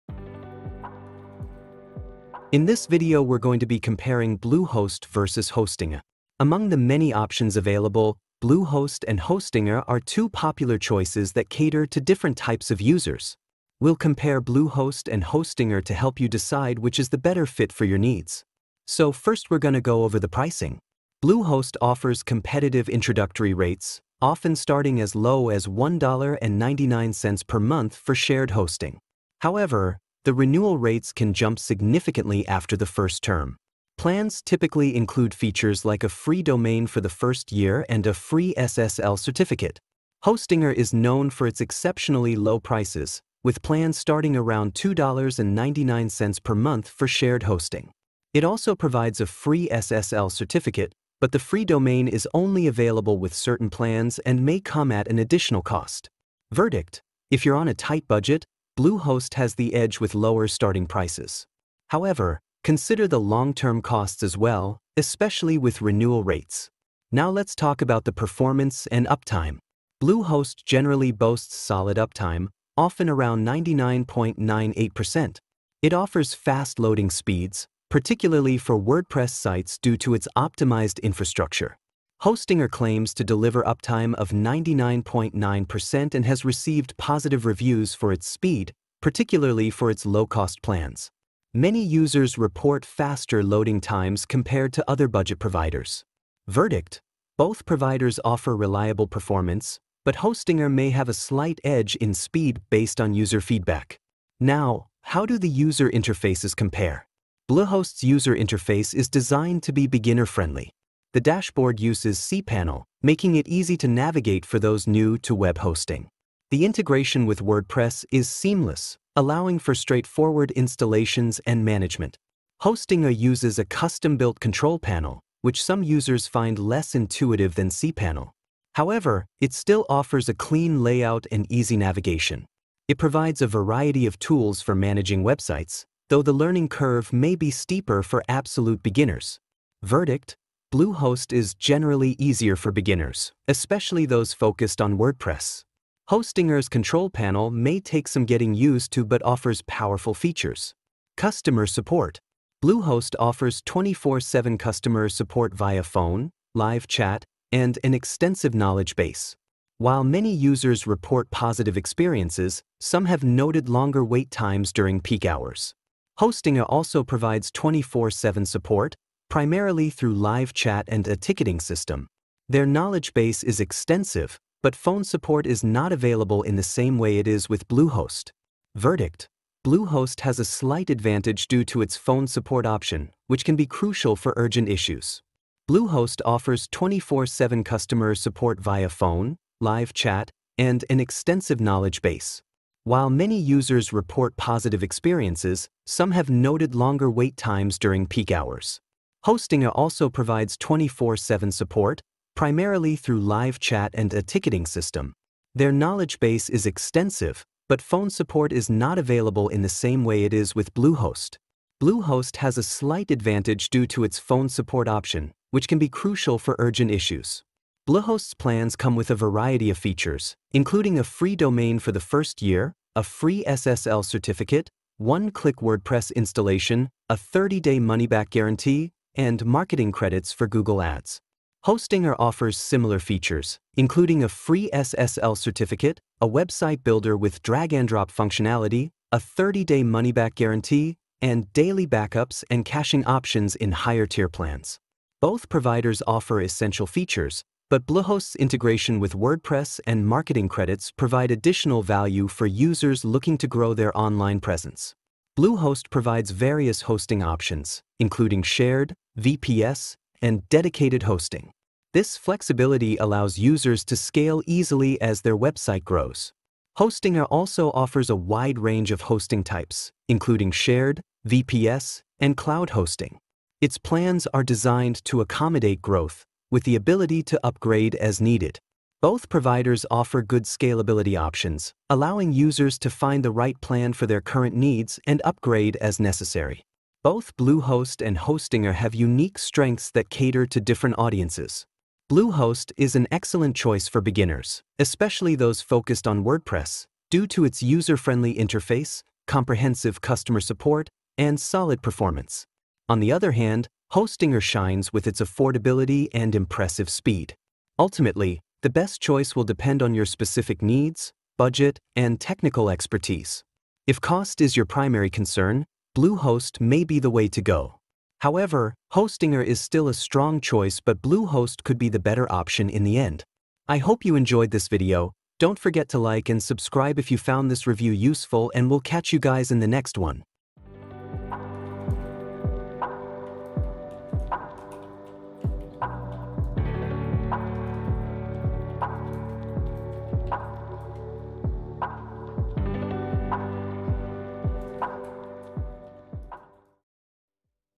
Bluehost vs Hostinger 2024: The Ultimate Showdown!
✅ Hosting Discounts When looking at Bluehost vs. Hostinger, it's helpful to check out what each one brings to the table. In a Bluehost review, many users like its user-friendly setup and reliable customer support, making it a solid choice for beginners. On the other hand, Hostinger often gets positive feedback for its affordable pricing and fast loading times, which is great for those on a budget. Both are considered among the best web hosting options, but your choice might depend on whether you want more support and features from Bluehost or cheaper rates from Hostinger. Each has its own strengths, so it really comes down to what fits your needs best!